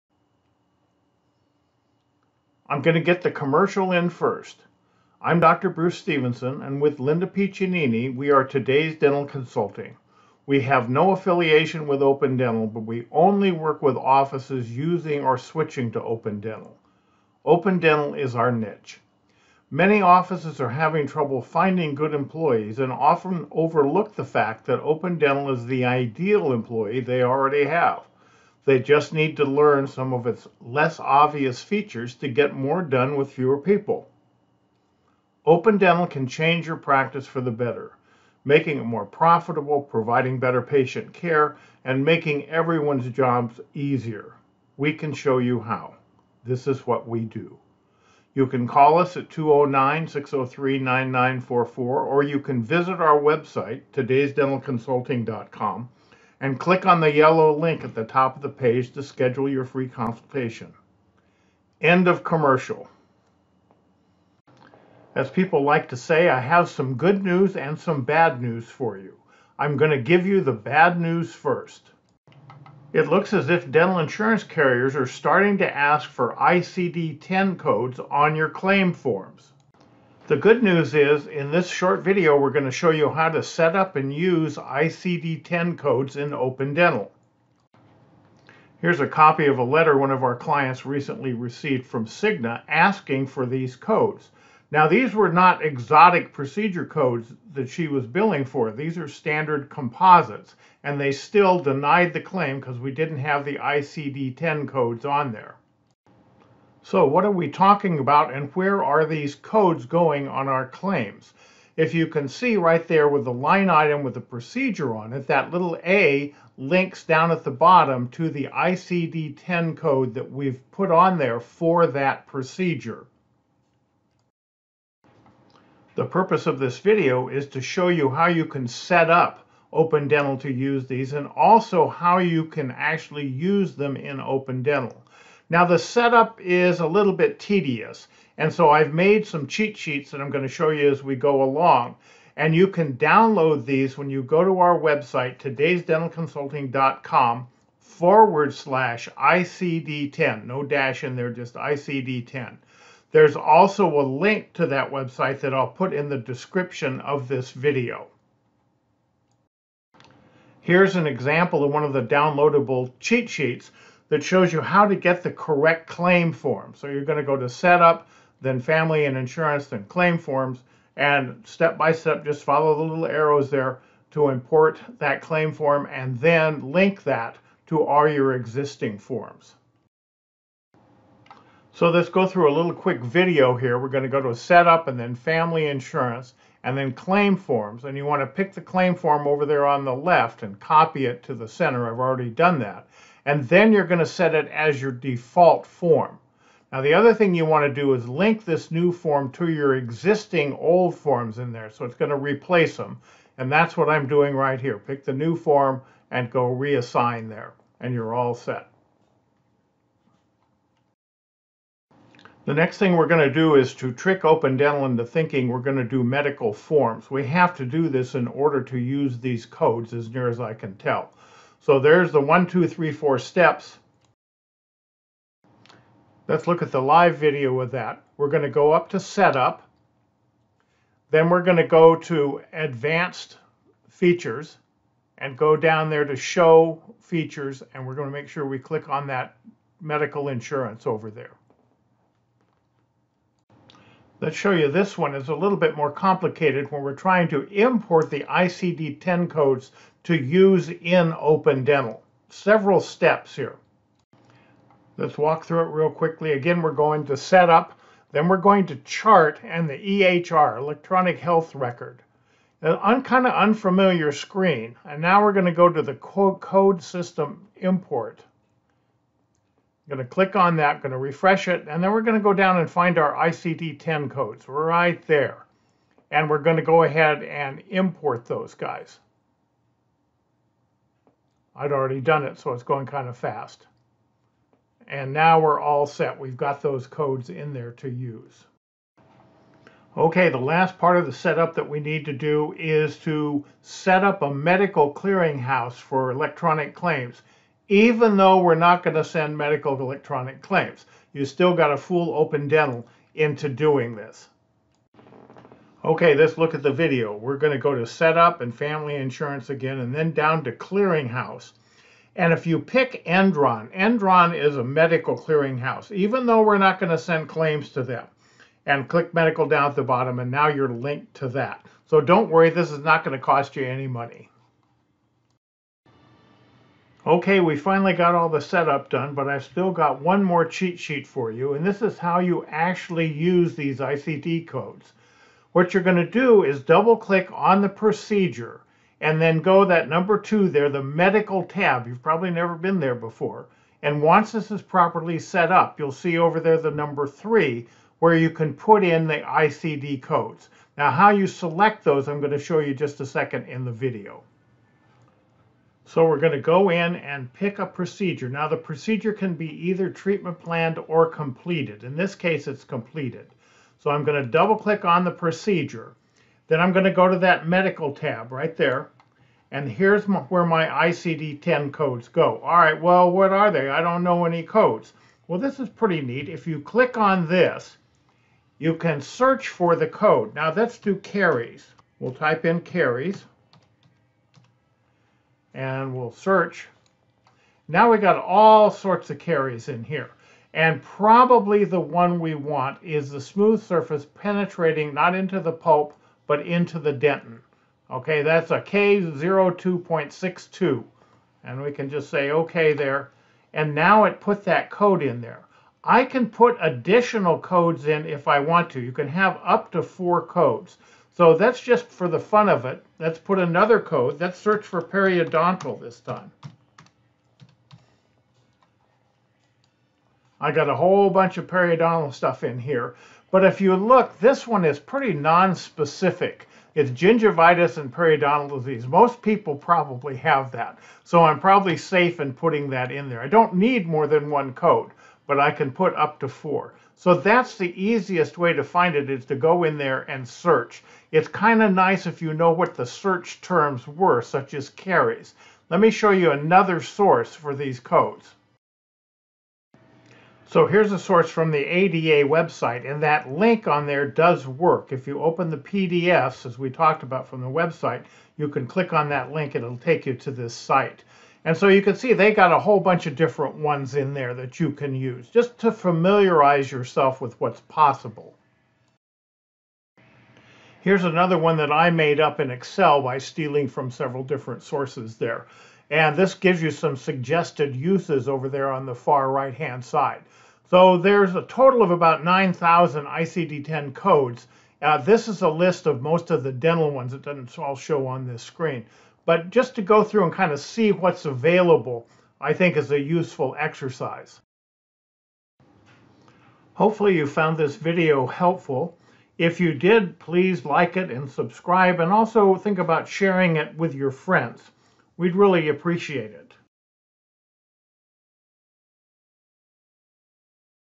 Dental Insurance Carriers Are Starting to Require ICD-10 Codes on Claims! Here is How to Do It in O
We are now seeing some dental insurance claims rejected for lack of ICD-10 codes!  This video show how to setup and use these codes in Open Dental software.